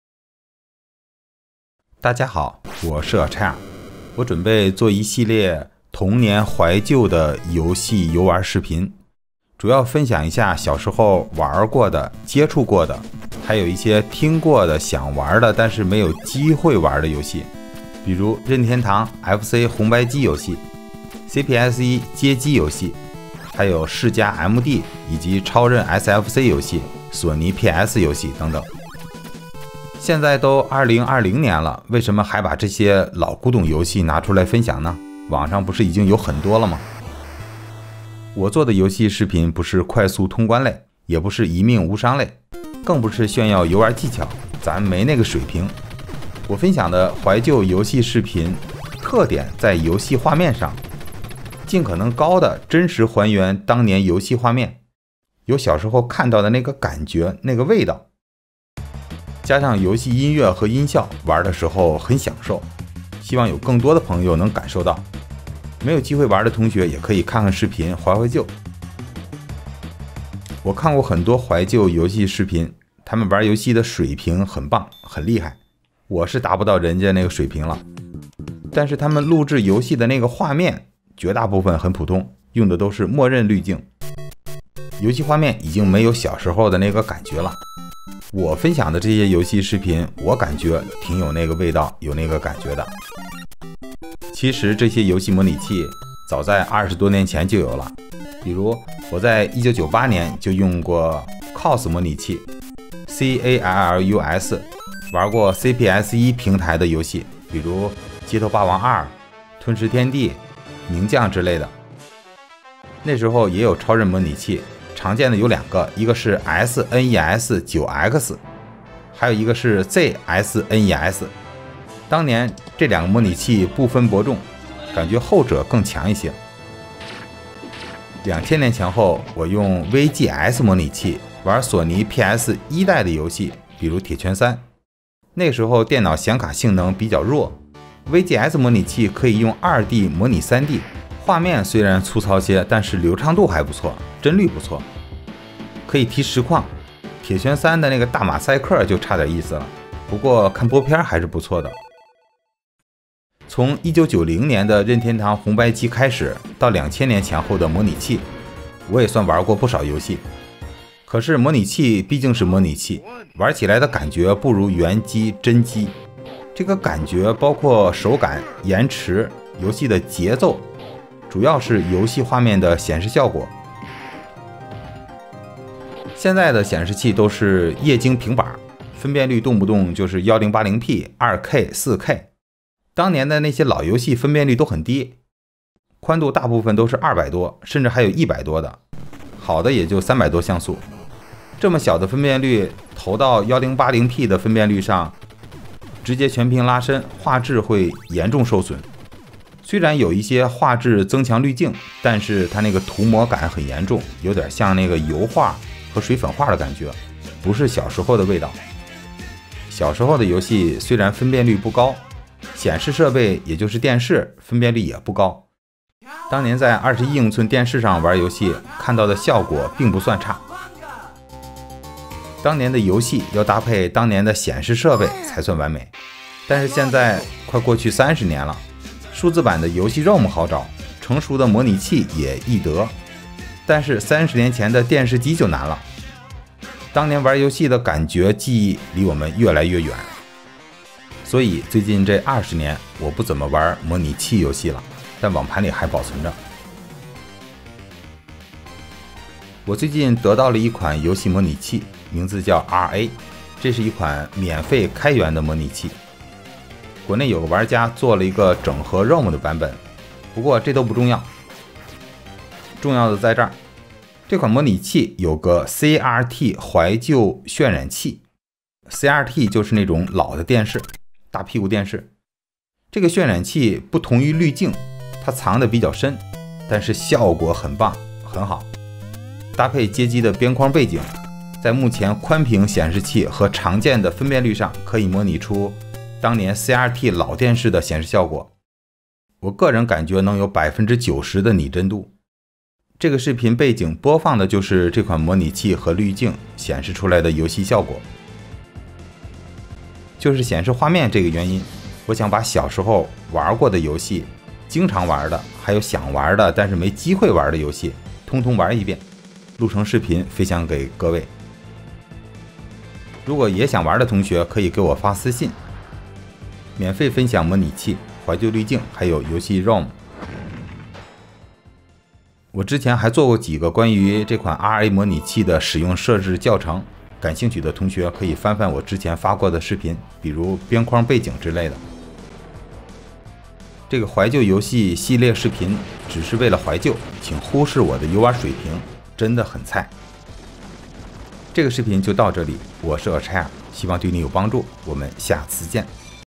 80后街机、游戏机童年怀旧经典游戏游玩（任天堂 索尼 世嘉等）
大家好，我是achair，我准备做一系列“童年怀旧游戏游玩视频”，主要分享小时候玩过的、接触过的，还有一些听过、想玩、但没机会玩过的游戏。比如：任天堂FC红白机游戏，CPS1街机游戏，世嘉MD和超任SFC游戏，索尼PS游戏等等。